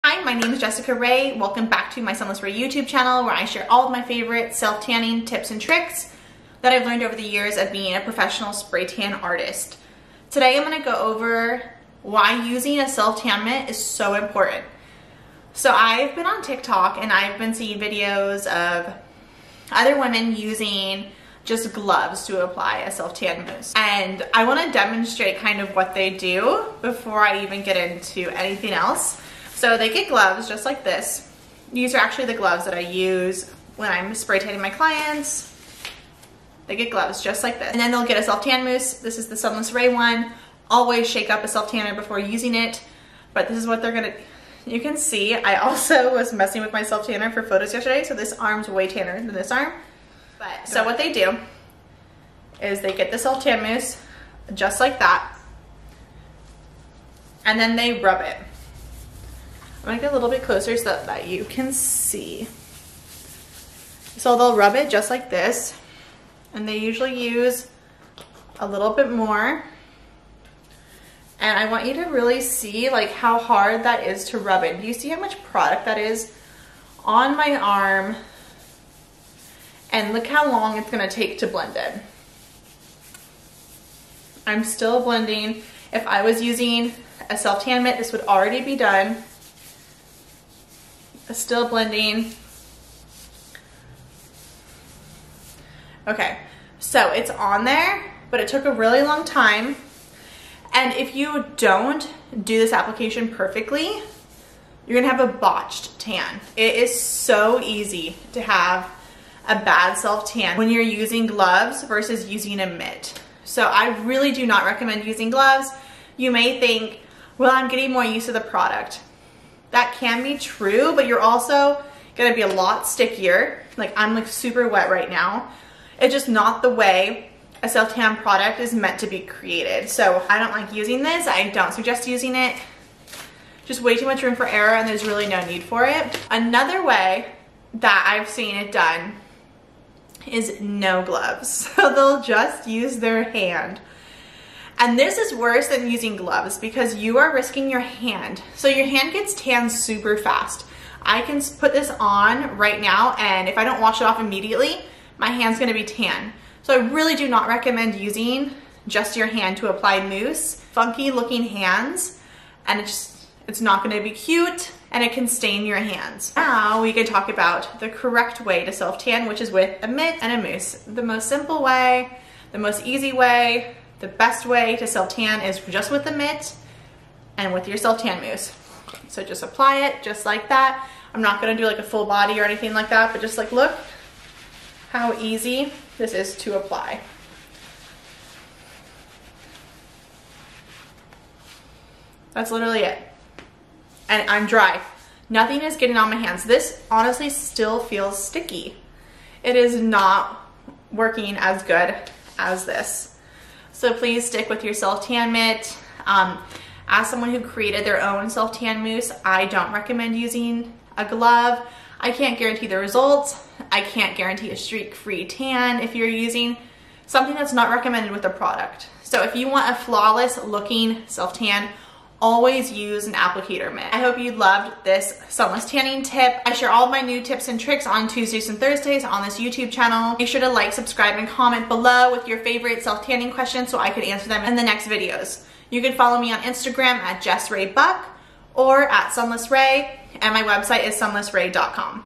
CAN I USE GLOVES TO SELF TAN? | TIPS AND TRICKS BY A PRO SPRAY TAN ARTIST
In this video sunless tanning expert, Jessica Rae owner of Sunless Rae, shares why you should NEVER use gloves or your hands to apply self tan mousse. If you do you risk a bad fake tan with an uneven and streaky application. The only way to have the best self tan is to use a self tan mitt.

Follow her on Instagram @jessraebuck and @sunlessrae